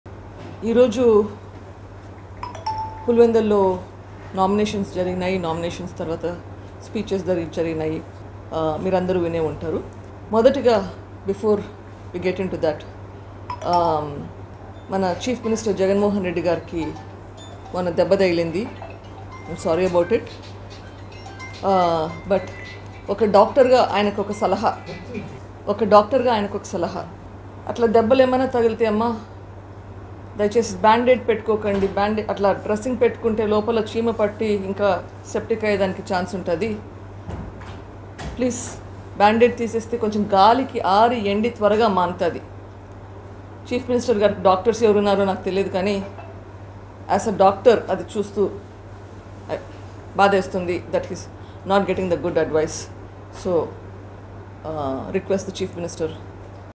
ఓడిపోవడానికి రెడీగా ఉండు జగన్.. | YS Sunitha Mass Warning to CM Jagan | ManamTvLive
ఓడిపోవడానికి రెడీగా ఉండు జగన్..
సునీత వార్నింగ్..